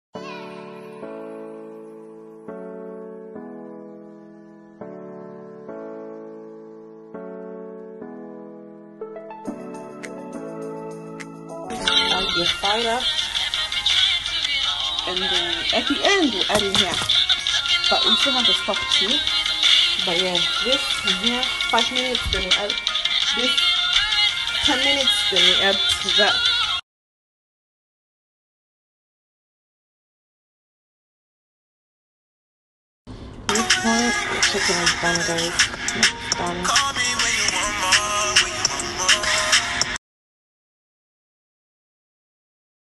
Incredible Chicken Curry| Best Ever Chicken stew | Simple Easy Delicious
Very simple recipe screenshot the ingredients list.
Lovely with any starch - my go to is nsima, sadza, pap or white rice to soak up the flavour of the stew.
Try it out and let me know how you go.